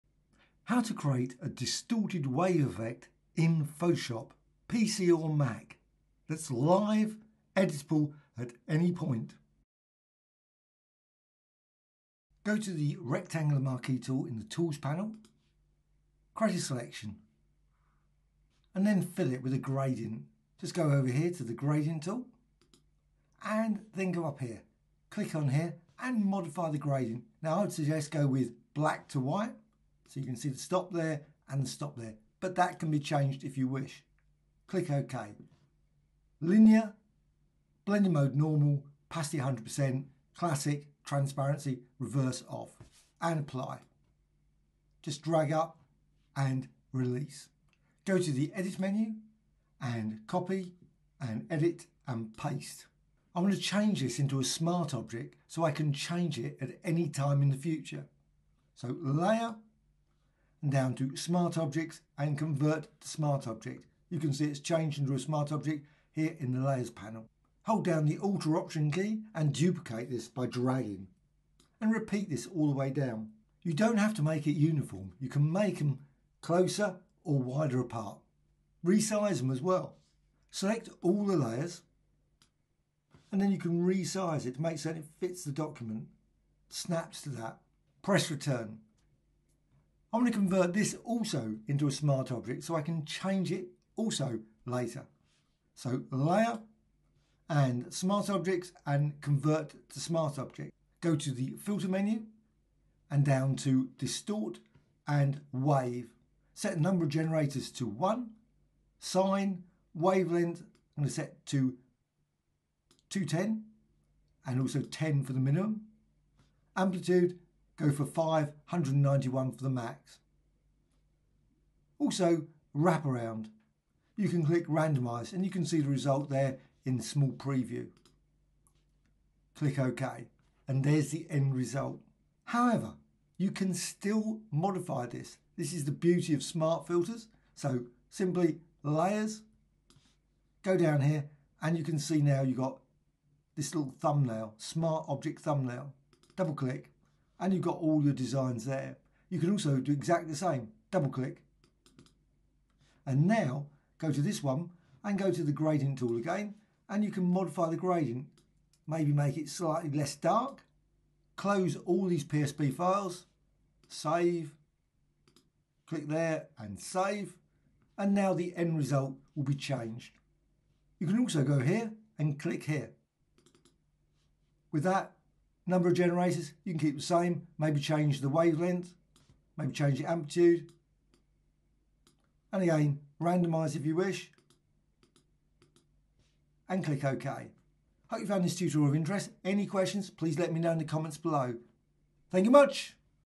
Photoshop : Wave Gradient Distortion Effect HOW TO
How to create 100s of great wave designs using the wave filter in Photoshop combined
00:00 - Examples
00:16 - Selection
00:42 - Apply gradient
01:02 - Smart object
01:47 - Wave
02:23 - Edit smart object